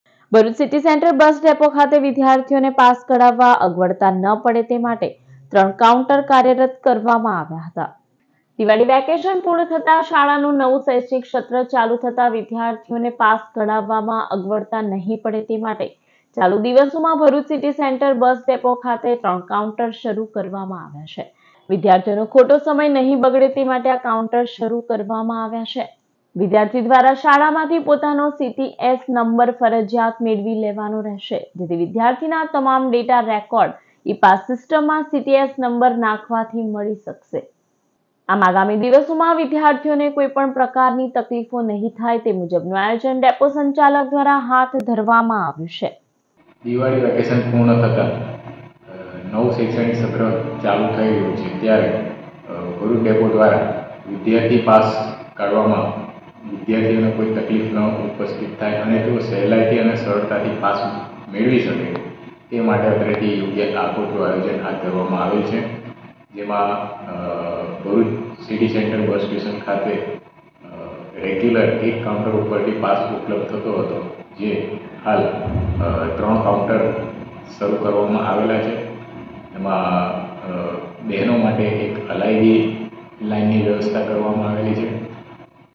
ભરૂચ સિટી સેન્ટર બસ ડેપો ખાતે વિદ્યાર્થીઓને પાસ કઢાવવા માટે ત્રણ કાઉન્ટર કાર્યરત કરવામાં આવ્યા
નર્મદા સમાચાર
આપની પોતાની ચેનલ : ચેનલ નર્મદા